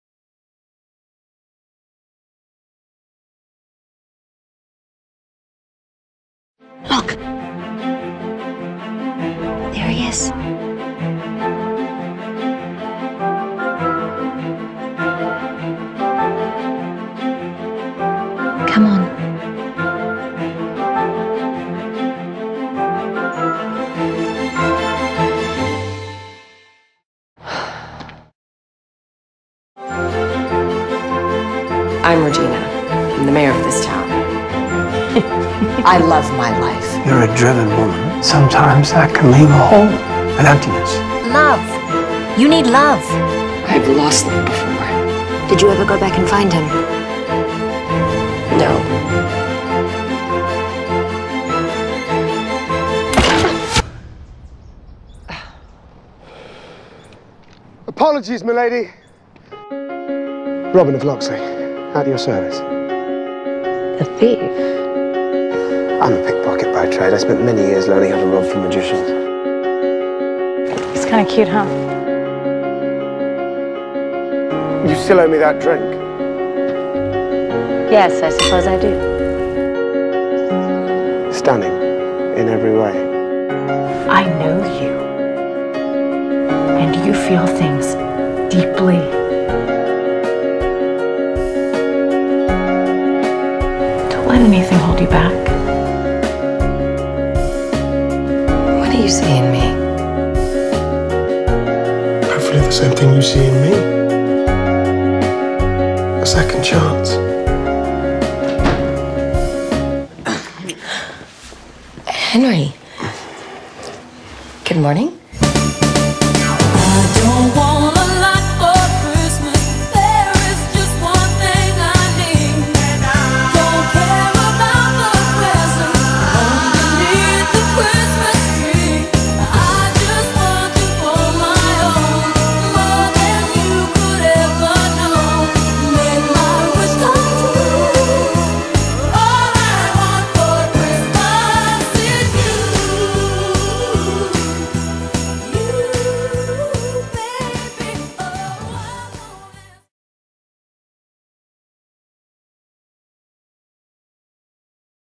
►A Soulmate for Christmas | Trailer
HD {1080p/720p} IS BETTER FOR YOUR EYES!

Hey, everyone! This video is my gift for the Outlaw Queen Christmas Advent Calendar. This is my 2nd year in a row participating, and it's always such a blast seeing what our talented fandom can create! This year, I decided to make a trailer for a Christmas-themed romantic comedy about Outlaw Queen :) Make sure to check out the website, it's amazing! Link is below. I hope everyone enjoys this!

I'd also like to give a HUGE shoutout to my girls Susanne (simplymaterial) and Karola for helping me with this video! I was a bit stressed and having a lot of vidders-block when making this haha, but these lovely ladies gave me tons of awesome ideas and FaceTimed me for hours until I finally came out with a decent result :) Thank you so much girls, YOU DA BEST :D I hope everyone has a wonderful Christmas/whichever holiday you celebrate! x